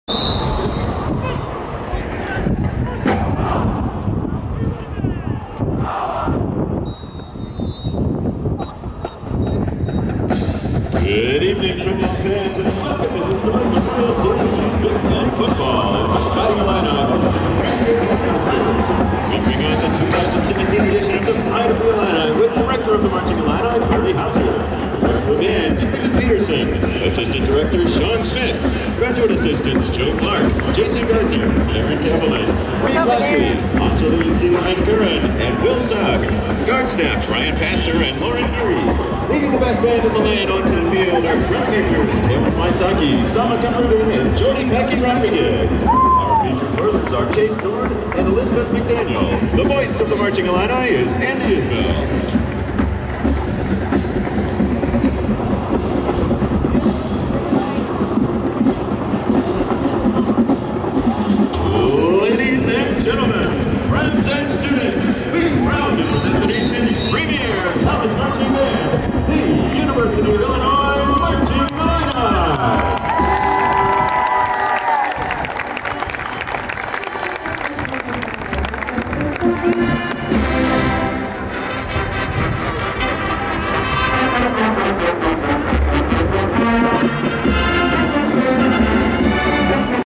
Marching Illini Band take field September 29, 2017.